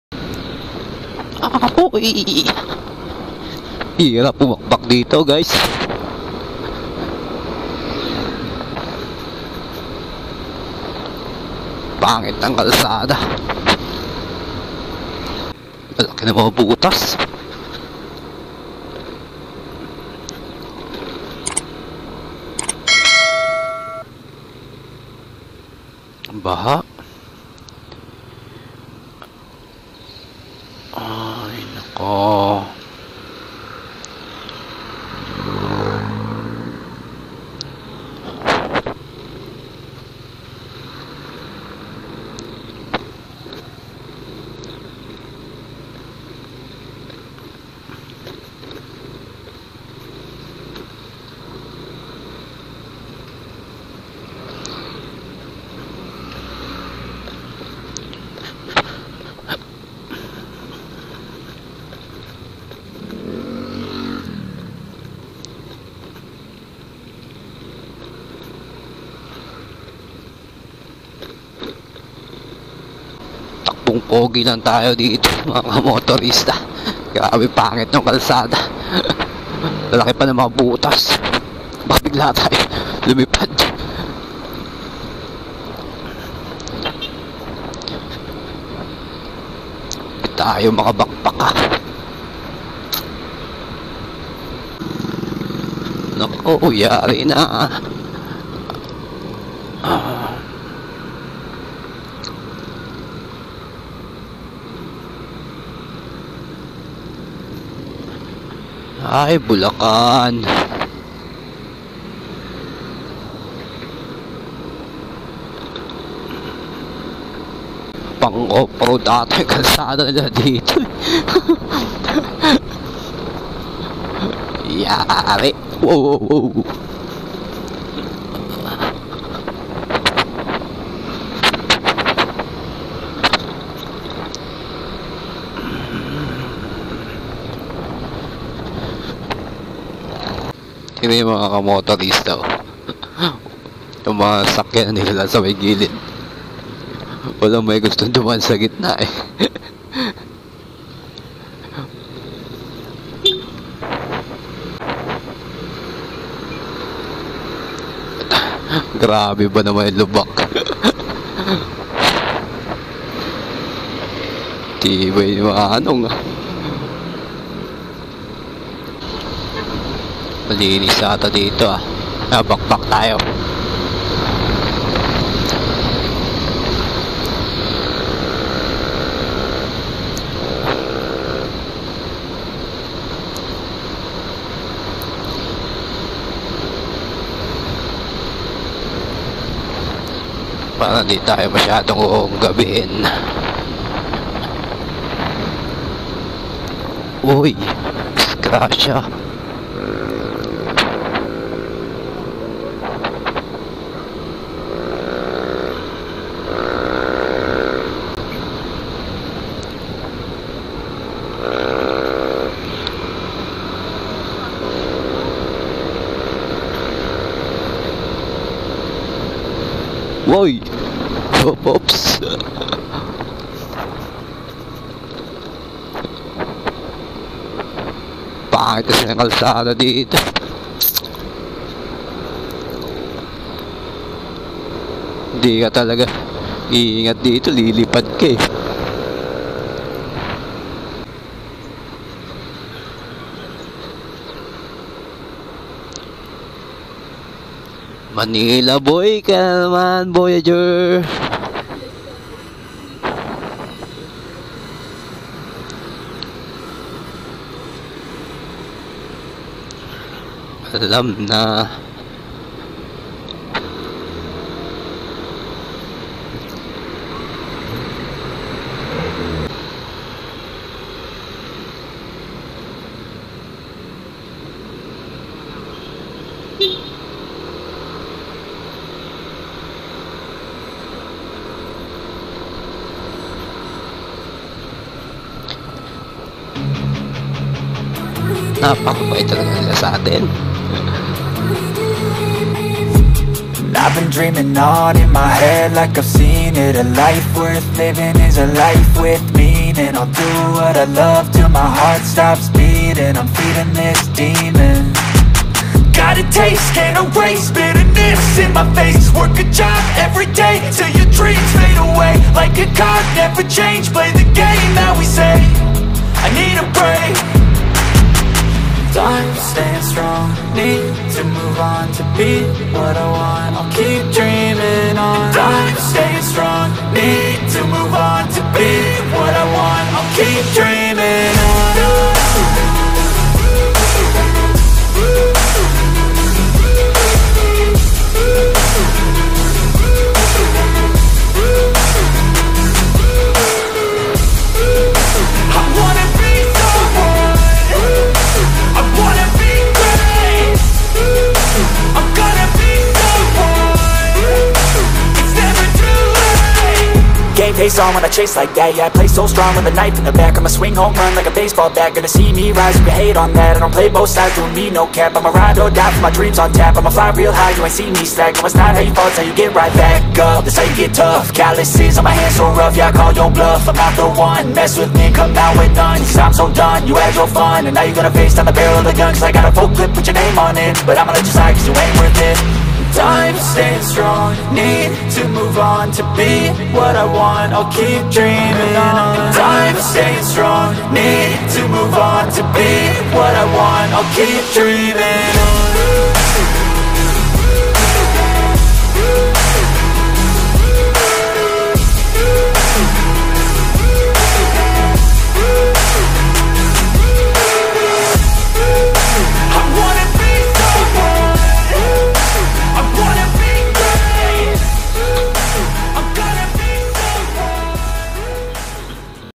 Pangasinan to Manila via Lubakan Road 😂😂
Lubakan Road Apalit, Bulacan, Valenzuela.. Grabi yung lubak dito, kung bakbak ka ng bakbak dito mabulaga ka na lang lilipad ka 😂